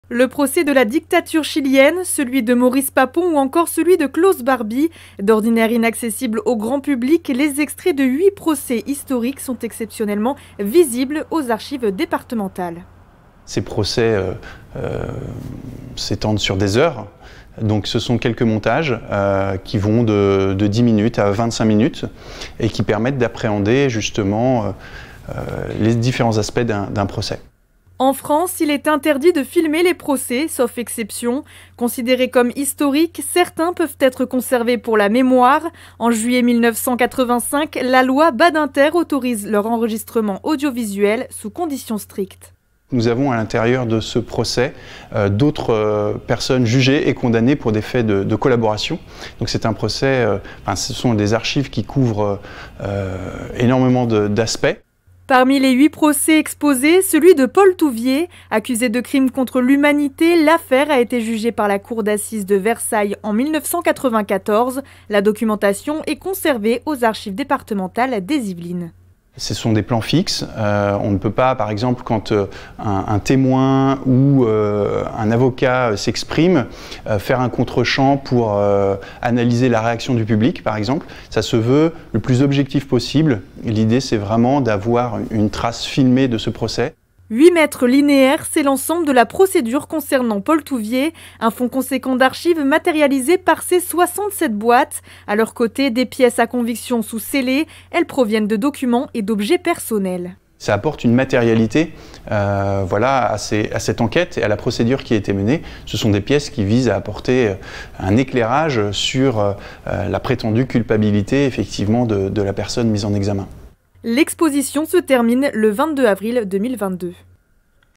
Yvelines | Des procès historiques exposés aux archives départementales
" Filmer les procès, un enjeu social : de Nuremberg au génocide des Tutsi au Rwanda" . Une exposition qui présente les extraits de huit procès historiques, filmés pour la mémoire. Créée par les archives nationales en 2020. Elle est visible actuellement aux archives départementales des Yvelines.